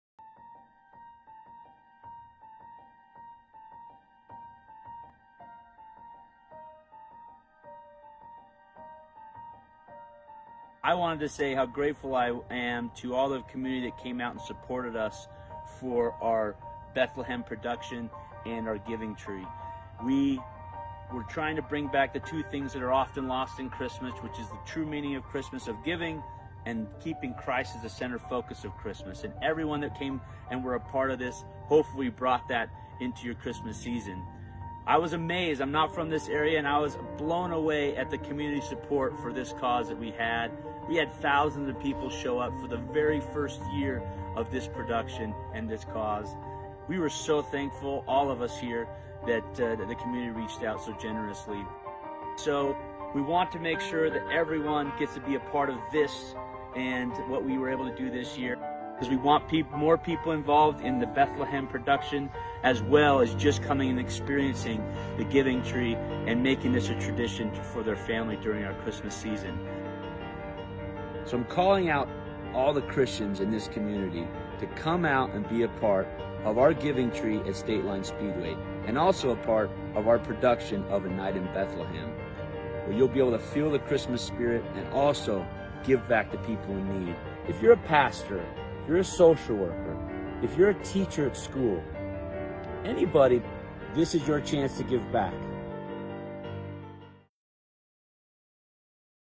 A Night In Bethlehem in Post Falls Idaho
A Night in Bethlehem is a free event for the community to celebrate the true meaning of Christmas and give guests an idea of what it was like on the day that our Savior was born. Walk the streets of Bethlehem then watch the live nativity. Stop by the largest giving tree in the northwest to help a family or organization from the community.